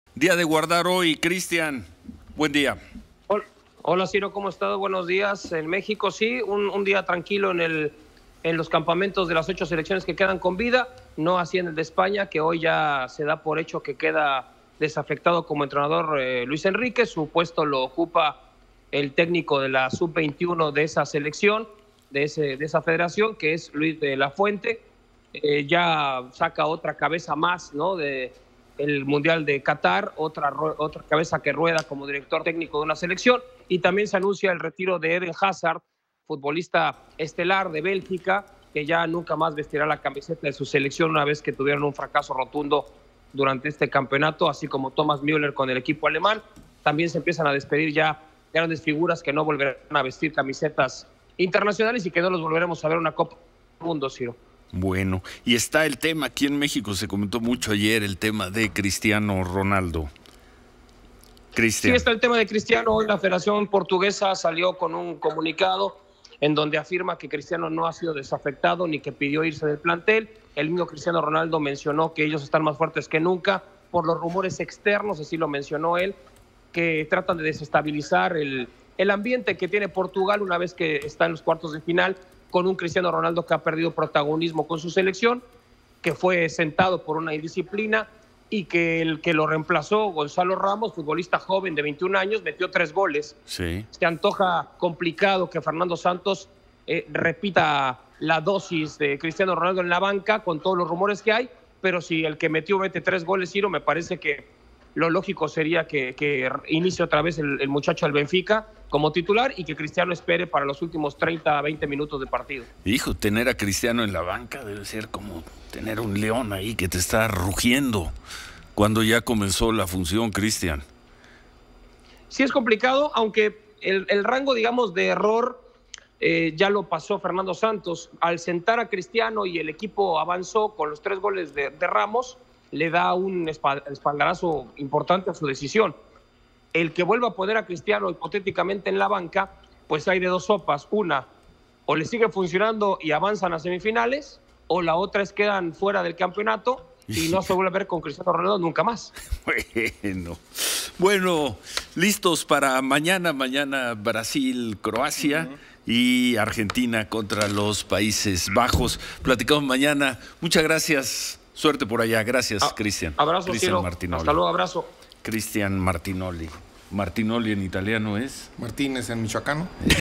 Martinoli desde Qatar 2022: Guardarse a Cristiano Ronaldo es la riesgosa apuesta de Portugal
Christian Martinoli comenta que Cristiano Ronaldo ha dicho estar más fuerte que nunca y los comentarios negativos entorno a la selección de Portugal sólo son intentos para desestabilizar al equipo. Martinoli explica que sentar en la banca –guardándolo para los últimos 30 minutos del partido– a un jugador como Cristiano Ronaldo tiene uno de dos resultados: seguir avanzando a las semifinales o quedar eliminados y probablemente jamás jugar con Cristiano Ronaldo de nuevo.

Grupo Fórmula